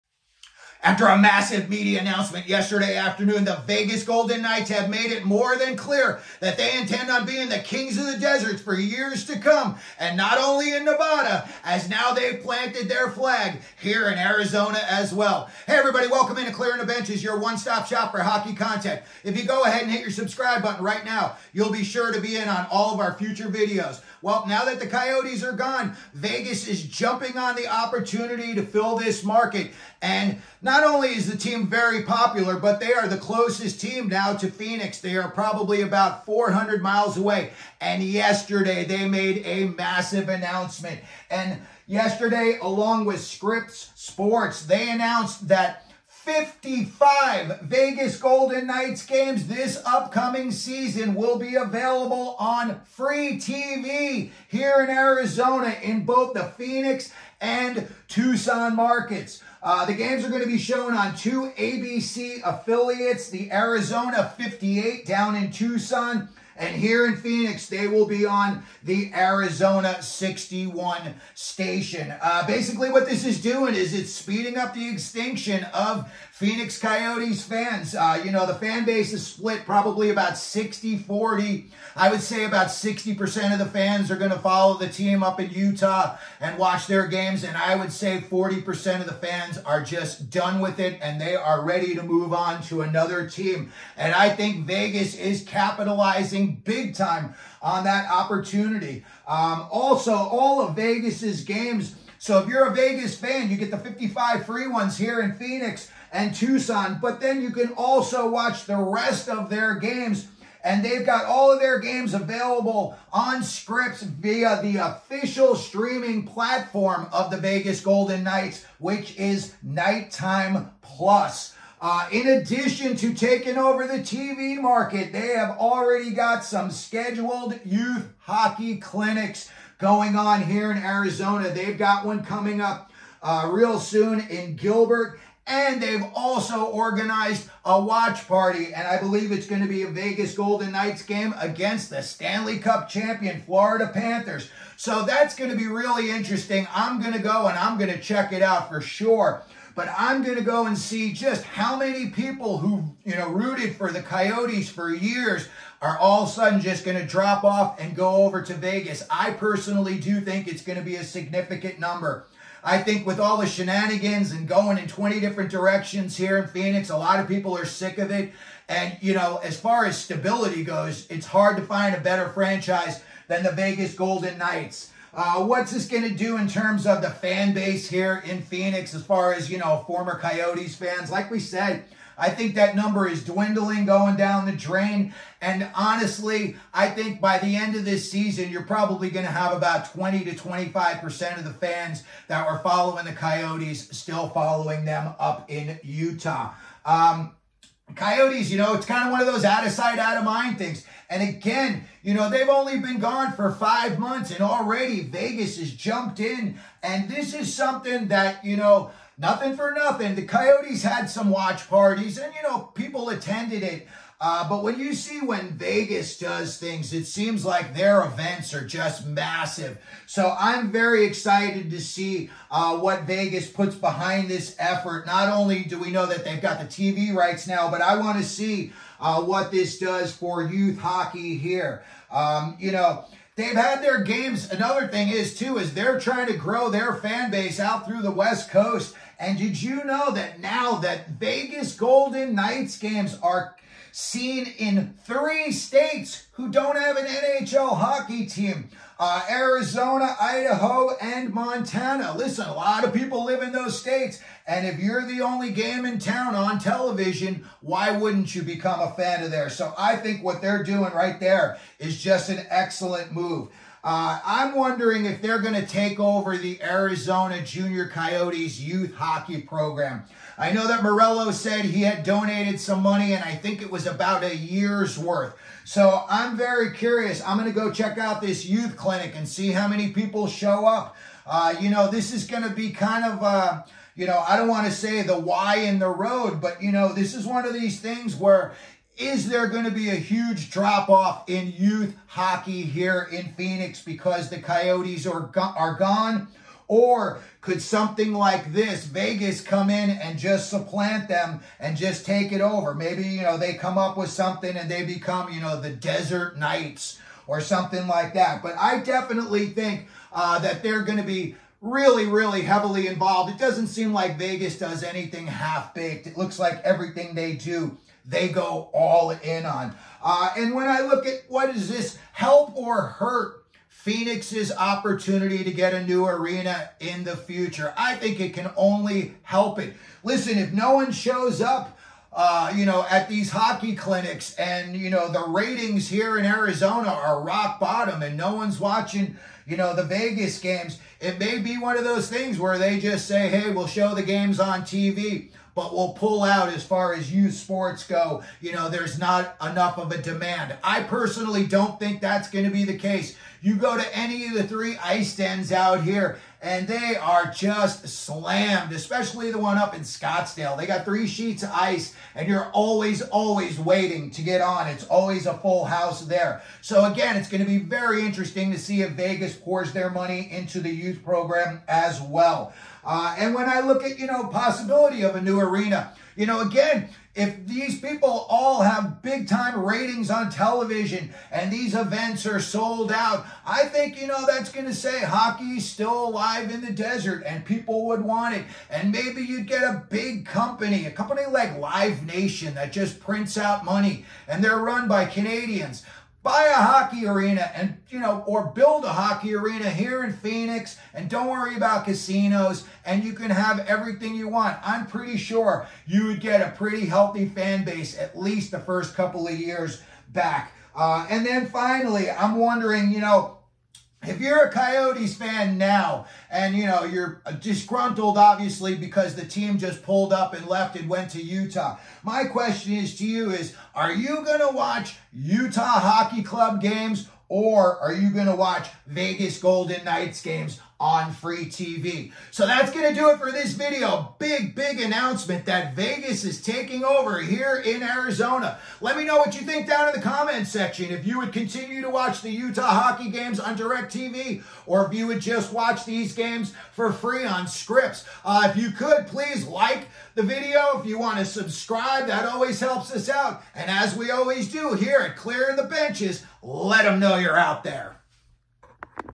VEGAS GOLDEN KNIGHTS TAKE OVER ARIZONA TV MARKET
VEGAS (September 26, 2024) – The Vegas Golden Knights and Scripps Sports announced today, September 26, 55-game broadcast schedules that will bring Golden Knights hockey to fans in the Phoenix and Tucson markets on Arizona 61 and Arizona 58, respectively.

Fans throughout the state of Arizona also will have the opportunity to watch every Scripps Sports VGK broadcast through KnightTime+, the official streaming platform of the Vegas Golden Knights.

“We are thrilled to expand the Realm of fans who can watch our local broadcast and appreciate our partners at Scripps Sports for helping make this a reality,” said Vegas Golden Knights President and CEO Kerry Bubolz. “Having our games available for free over the air, or through KnightTime+, is a win for hockey fans in Arizona.”

Fans in Arizona will be able to follow one of the NHL’s most successful franchises in the Golden Knights, who have qualified for the playoffs in six of their seven seasons, reached the penultimate round of the Stanley Cup Playoffs four times and won the Stanley Cup in 2023.

“As the intense fandom around hockey grows, we know how important it is for the Vegas Golden Knights to reach their expanding fanbase on every device and platform,” said Scripps Sports President Brian Lawlor. “These new Scripps Sports-produced local broadcasts, combined with Knight Time+, will make sure that fans in the Phoenix and Tucson markets never have to miss a game.”

The Golden Knights will celebrate these new broadcasts by hosting youth ice hockey and ball hockey clinics and a VGK watch party on Saturday, October 19 at AZ Ice in Gilbert, AZ. VGK youth hockey staff and members of the VGK Cast will be in attendance.

Events will include ice hockey clinics for ages 5 to 8 (12:30 p.m. PT) and ages 9 to 12 (1:30 p.m. PT), a ball hockey clinic for all ages (12:30 p.m. PT) and a watch party at Park Tavern & Market for the Golden Knights game at Florida (3 p.m. PT). Registration is required for the on-ice clinics and is available here.

This marks the second season of the Golden Knights’ partnership with Scripps Sports to televise non-nationally exclusive games for free over the air, on cable or with a satellite subscription. In addition to Arizona, broadcasts are available in Southern Nevada (Vegas 34), Northern Nevada (NSN), Idaho (Boise 6 and Twin Falls 6) and Montana (MTN).

This is also the second year that the Golden Knights and Scripps Sports have partnered on KnightTime+, the Vegas Golden Knights’ official streaming platform that carries all locally aired games for fans within the team’s television territory.

VGK 2024-25 TELEVISION & STREAMING SCHEDULE IN ARIZONA

Wed., Oct. 9 vs. Colorado – TNT, MAX
Fri., Oct. 11 vs. St. Louis – Arizona 61/Arizona 58, KnightTime+
Sun., Oct. 13 vs. Anaheim – Arizona 61/Arizona 58, KnightTime+
Tue., Oct. 15 at Washington – Arizona 61/Arizona 58, KnightTime+
Thurs., Oct. 17 at Tampa Bay – Arizona 61/Arizona 58, KnightTime+
Sat., Oct. 19 at Florida – Antenna TV, KnightTime+
Tue., Oct. 22 vs. Los Angeles – ESPN, ESPN+
Fri., Oct. 25 vs. Ottawa – Arizona 61/Arizona 58, KnightTime+
Sat., Oct. 26 vs. San Jose – Antenna TV, KnightTime+
Mon., Oct. 28 vs. Calgary – Arizona 61/Arizona 58, KnightTime+
Wed., Oct. 30 vs. Los Angeles – TNT, truTV, MAX
Sat., Nov. 2 vs. Utah – Arizona 61/Arizona 58, KnightTime+
Wed., Nov. 6 at Edmonton – Arizona 61/Arizona 58, KnightTime+
Fri., Nov. 8 at Seattle – Arizona 61/Arizona 58, KnightTime+
Mon., Nov. 11 vs. Carolina – Arizona 61/Arizona 58, KnightTime+
Wed., Nov. 13 at Anaheim – KnightTime+
Fri., Nov. 15 at Utah – Arizona 61/Arizona 58, KnightTime+
Sun., Nov. 17 vs. Washington – Arizona 61/Arizona 58, KnightTime+
Wed., Nov. 20 at Toronto – Arizona 61/Arizona 58, KnightTime+
Thurs., Nov. 21 at Ottawa – Antenna TV, KnightTime+
Sat., Nov. 23 at Montreal – Arizona 61/Arizona 58, KnightTime+
Mon., Nov. 25 at Philadelphia – Arizona 61/Arizona 58, KnightTime+
Wed., Nov. 27 at Colorado – TNT, MAX
Fri., Nov. 29 vs. Winnipeg – KnightTime+
Sat., Nov. 30 vs. Utah – Arizona 61/Arizona 58, KnightTime+
Tue., Dec. 3 vs. Edmonton – ESPN+, Hulu
Wed., Dec. 4 at Anaheim – Arizona 61/Arizona 58, KnightTime+
Fri., Dec. 6 vs. Dallas – Arizona 61/Arizona 58, KnightTime+
Thurs., Dec. 12 at Winnipeg – KnightTime+
Sat., Dec. 14 at Edmonton – Antenna TV, KnightTime+
Sun., Dec. 15 at Minnesota – Arizona 61/Arizona 58, KnightTime+
Thurs., Dec. 19 vs. Vancouver – Arizona 61/Arizona 58, KnightTime+
Sat., Dec. 21 vs. Seattle – Arizona 61/Arizona 58, KnightTime+
Mon., Dec. 23 vs. Anaheim – Arizona 61/Arizona 58, KnightTime+
Fri., Dec. 27 at San Jose – Arizona 61/Arizona 58, KnightTime+
Sun., Dec. 29 vs. Calgary – Arizona 61/Arizona 58, KnightTime+
Tue., Dec. 31 vs. Montreal – KnightTime+